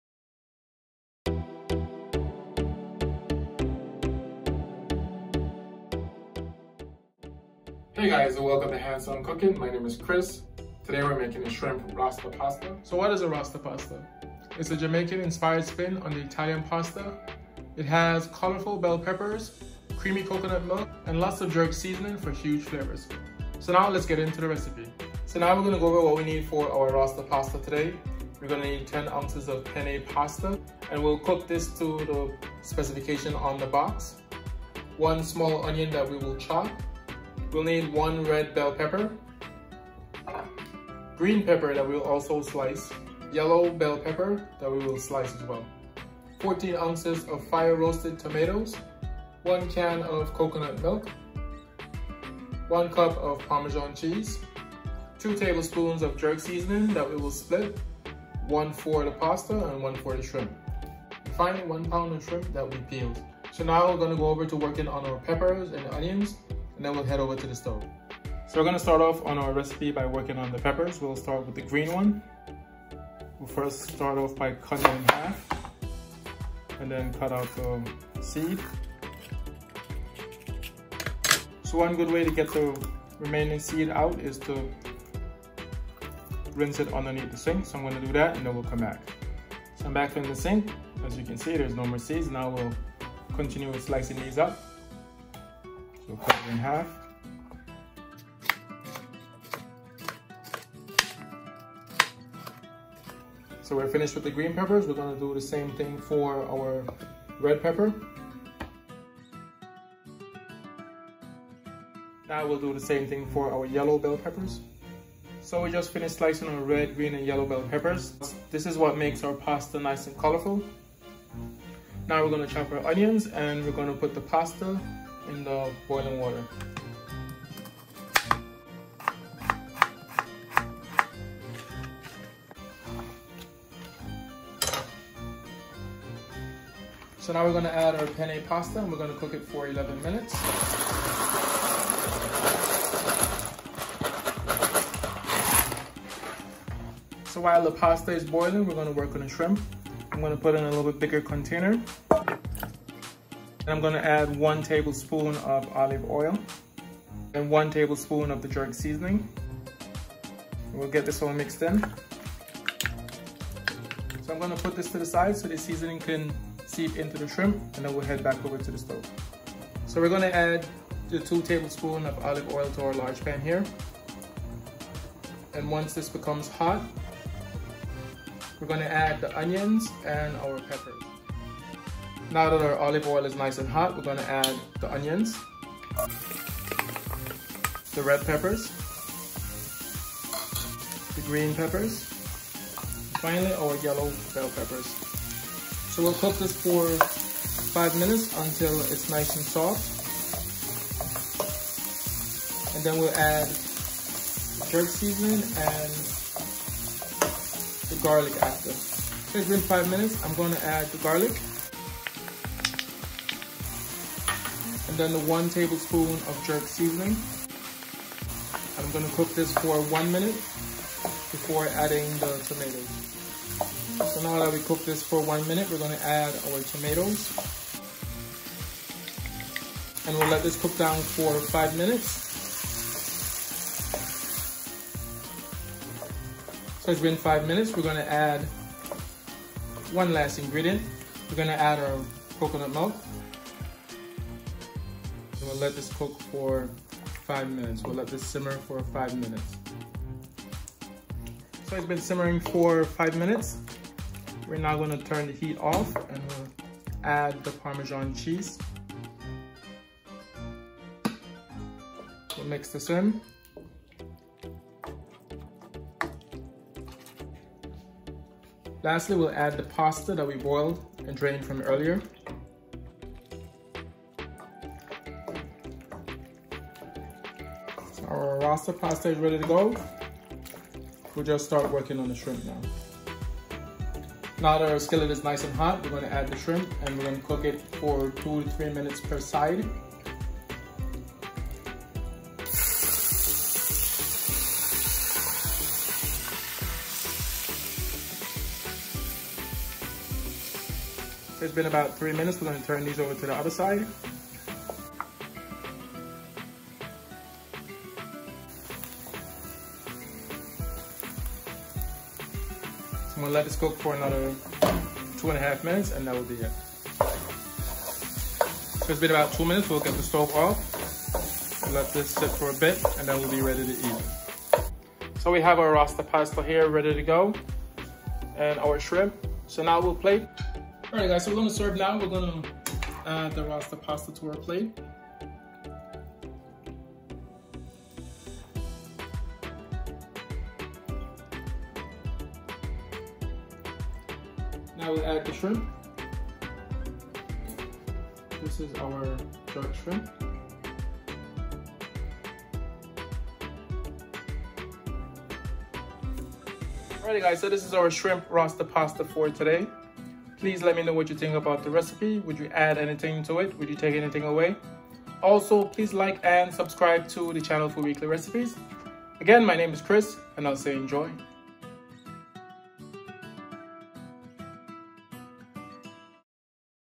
Easy Shrimp Rasta Pasta / Recipe /
Today we will be going step by step to show you; how to make a delicious Shrimp Rasta Pasta.  From start to finish.   @Handsoncooking

Prep Time - 10 mins    /  Cook Time- 20 mins

Ingredients
FOR THE RASTA PASTA
2 tablespoons olive oil or vegetable oil
1 small onion chopped
1 green bell pepper sliced
1 red bell pepper sliced
1 yellow bell pepper sliced
3 cloves garlic chopped
1 tablespoon Jamaican jerk seasoning
14 ounces fire roasted tomatoes
1 cup coconut milk heavy cream is an acceptable alternative, yet won’t bring that coconut flavor
1 cup of grated fresh Parmesan cheese or more as needed
10 ounces fettuccine pasta

FOR THE SHRIMP (optional)
1 pound shrimp peeled and deveined
1 tablespoon olive oil
1 tablespoon Jamaican jerk seasoning

Instructions
1. Heat 2 tablespoons olive oil in a large pan and add the onion and peppers. Cook them down about 5 minutes to soften.
2.  Add the garlic and 1 tablespoon Jamaican jerk seasoning. Cook for 1 minute, stirring.
3. Add the tomatoes and stir. Cook for 5 minutes to cook down the tomatoes.
4. Add the coconut milk and simmer for 5 minutes to thicken up. If it get too thick, swirl in a few tablespoons of water or chicken stock. Stir in the Parmesan cheese and remove from heat.
5. While the Rasta Pasta is simmering, cook the fettuccine noodles according to the package directions. Drain and toss with the sauce.

FOR THE SHRIMP
1. Toss the shrimp with 1 tablespoon olive oil and 1 tablespoon Jamaican jerk seasoning.
2. Heat a separate pan to medium heat and cook the shrimp 2-3 minutes per side, until pink and cooked through.
3. Serve the shrimp over the pasta. Garnish with fresh chopped parsley, spicy chili flakes, and extra grated Parmesan cheese

4. ENJOY!